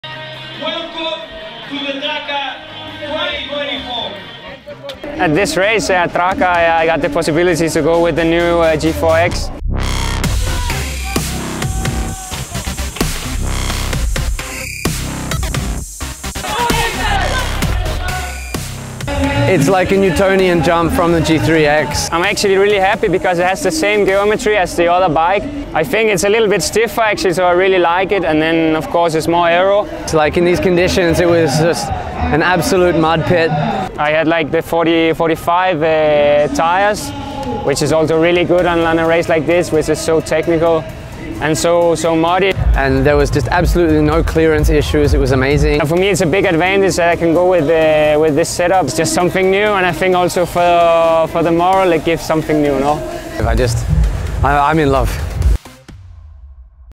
Traka 24 | Post race interview with Nathan Haas and Asbjørn Hellemose
Let’s heard the voice of the protagonists, Nathan Haas and Asbjørn Hellemose, right after the race. This Traka was special: because of the bad weather and of the mud along the route, because of the race debut of the G4-X.